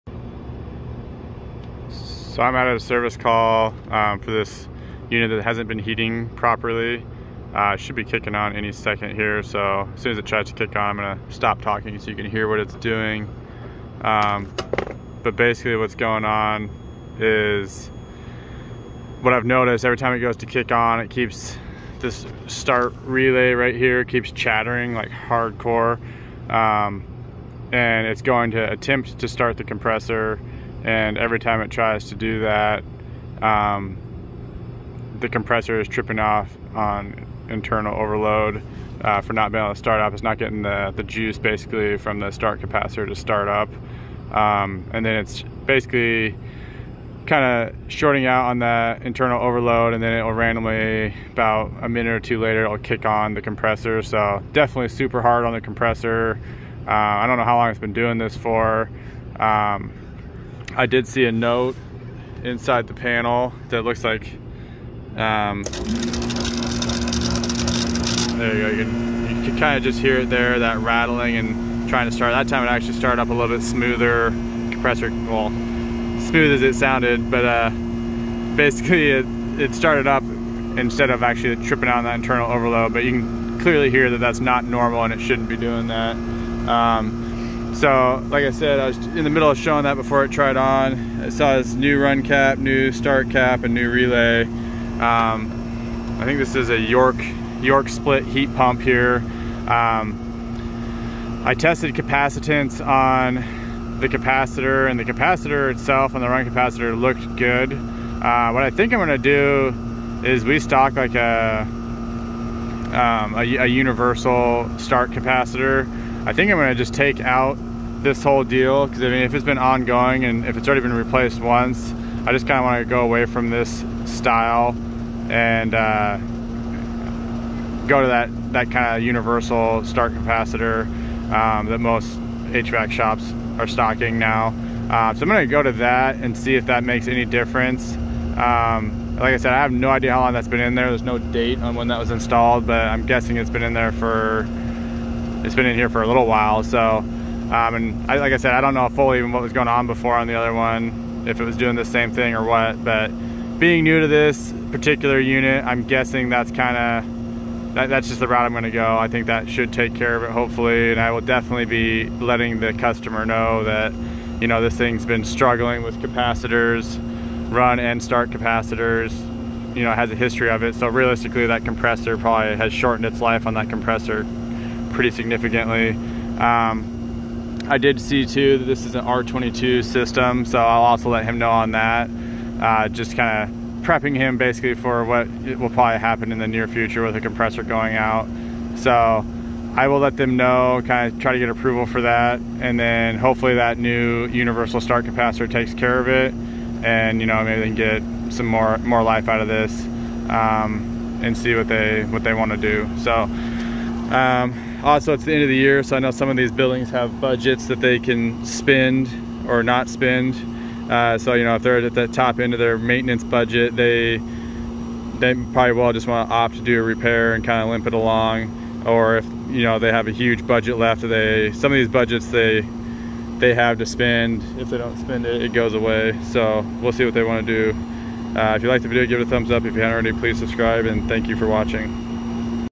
York Heat Pump Troubleshooting
Found the start capacitor went out and is damaging the compressor.